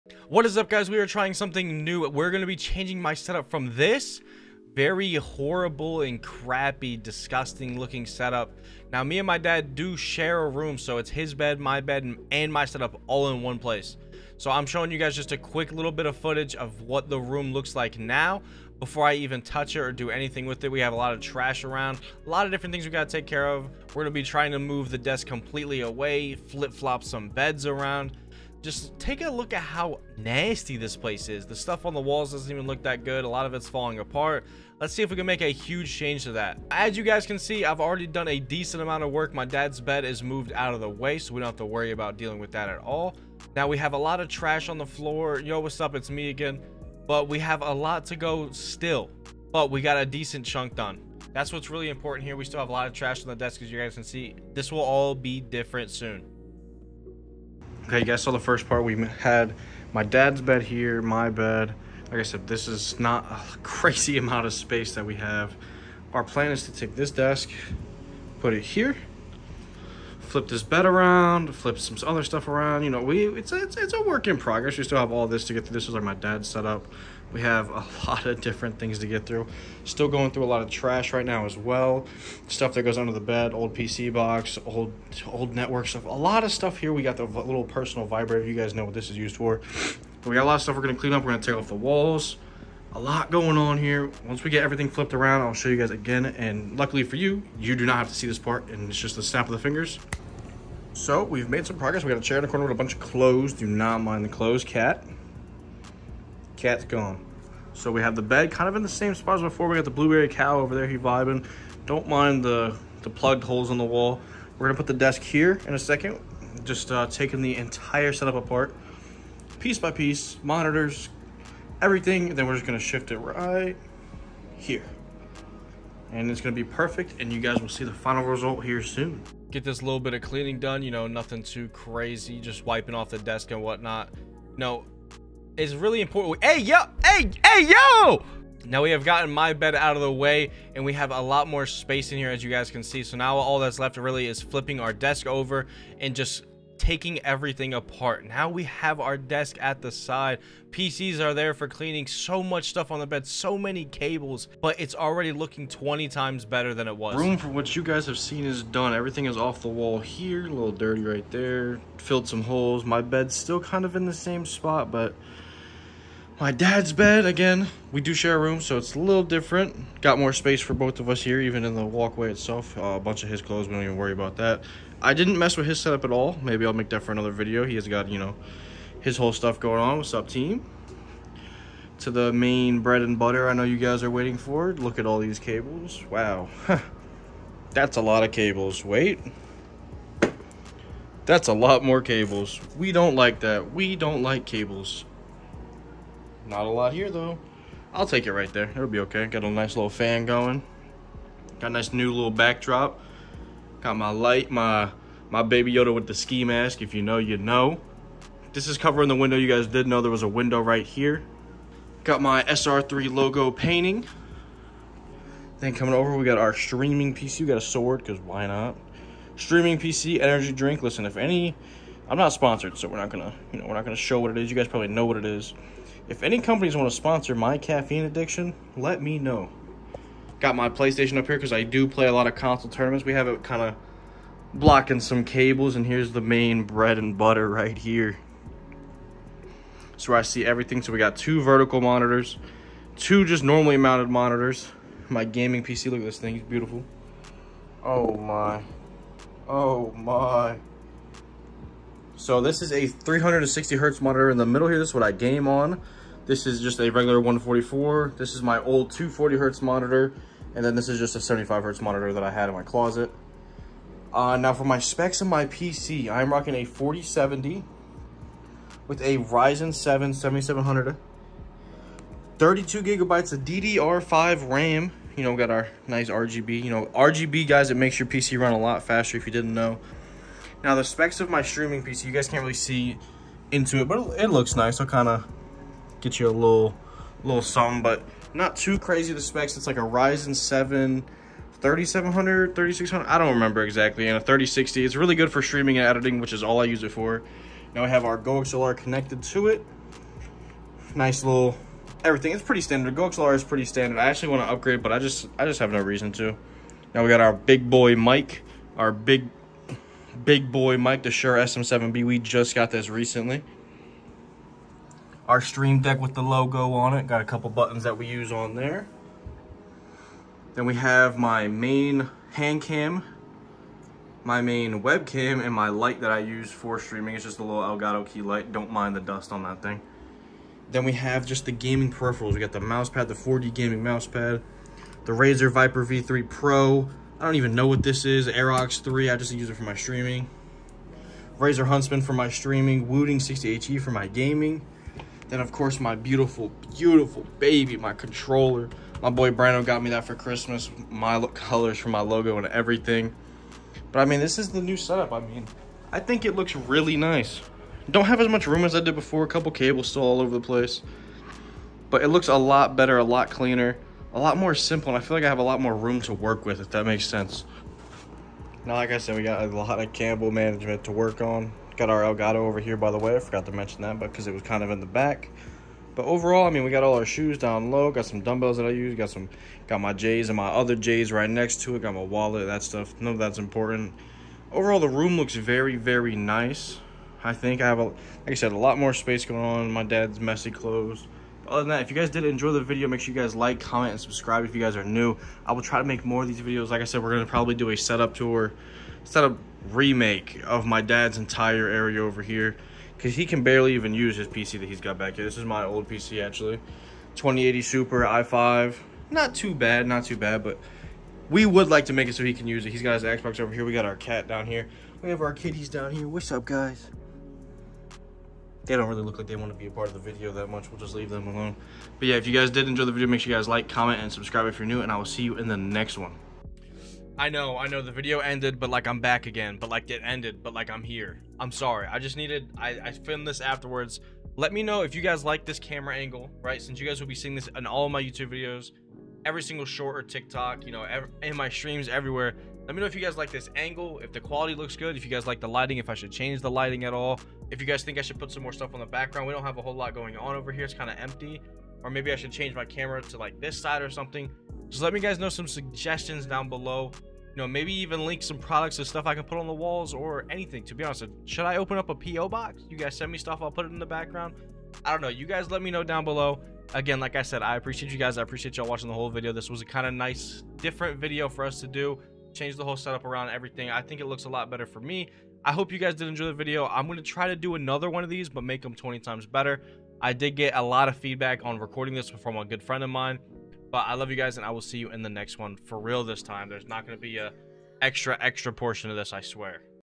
Transforming my Old MESSY Room into my *DREAM* Gaming Room!
Changing my old nasty room into my DREAM Setup